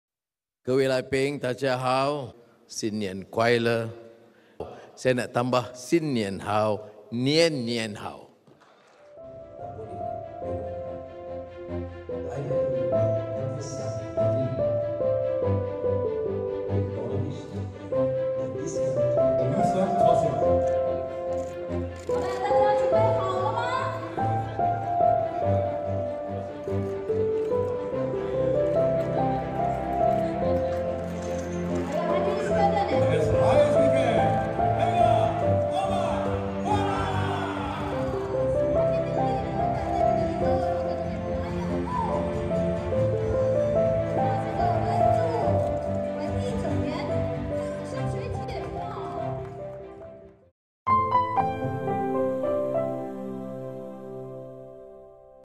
马来西亚总理安瓦尔出席华社活动秀中文拜年
当地时间2月15日，马来西亚总理安瓦尔于吉隆坡出席马来西亚中华总商会新春活动时，秀中文向来宾拜年。安瓦尔、马来西亚国会下议院议长佐哈里及多位部长还兴致勃勃参与马来西亚华人传统的新年活动——捞生。捞生是将各种色彩丰富的食材盛于盘中，并淋上桔子汁，参与者一起用筷子将食材搅拌并捞起，同时大喊“发啊”“旺啊”，表达新年祝福。